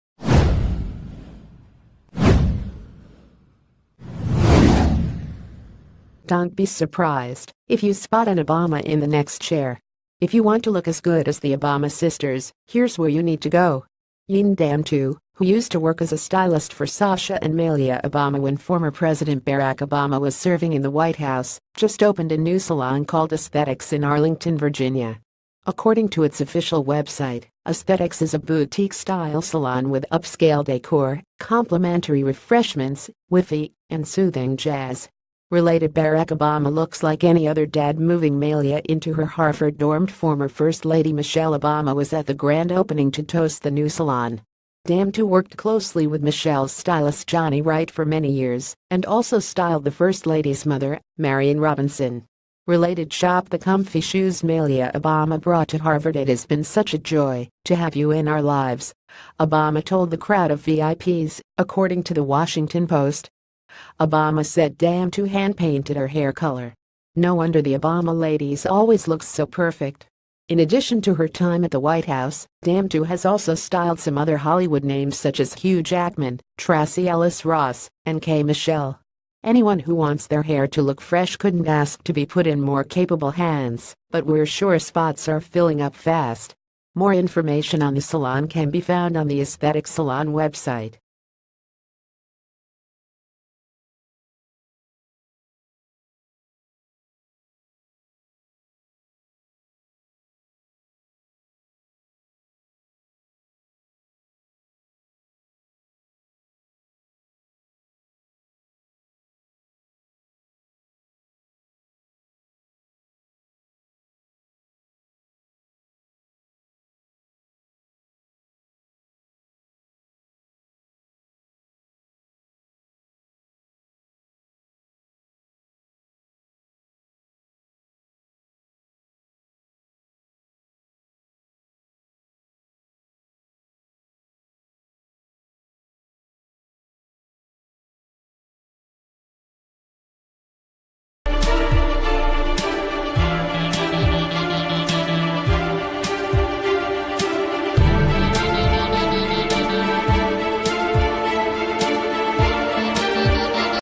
Breaking News | Get your hair done by sasha and malia obama's stylist at her new salon
Get your hair done by sasha and malia obama's stylist at her new salon

Dont be surprised if you spot an Obama in the next chair.  If you want to look as good as the Obama ...